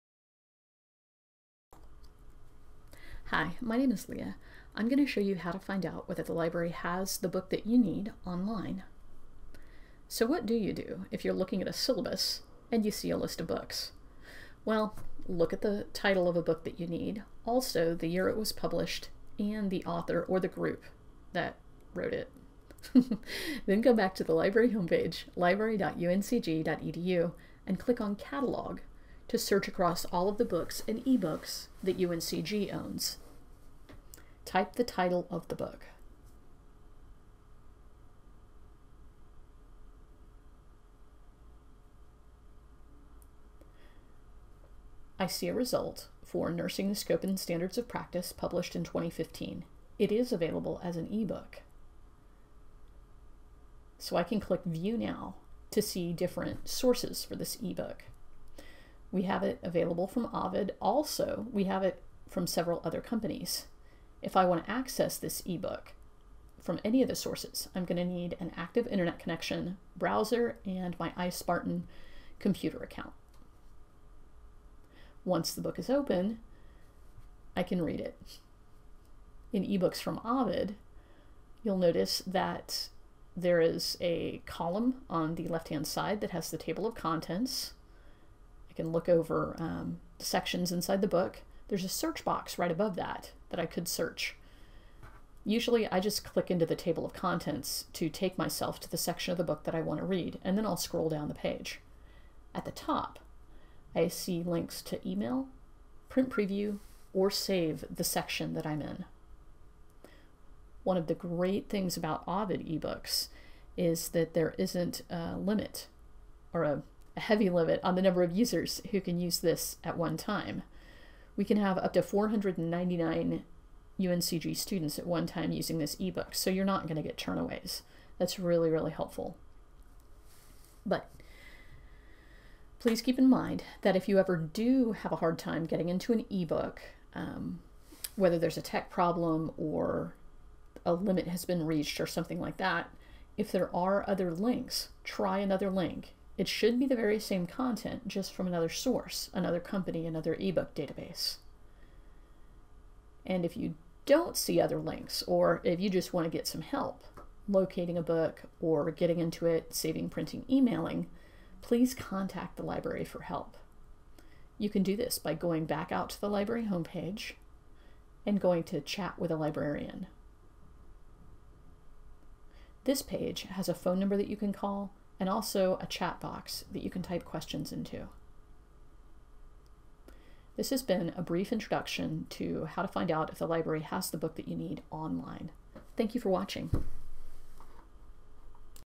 Find out if the UNCG Libraries have the book you need online
Demonstration of WorldCat Local search recorded April 2017. 3 minutes.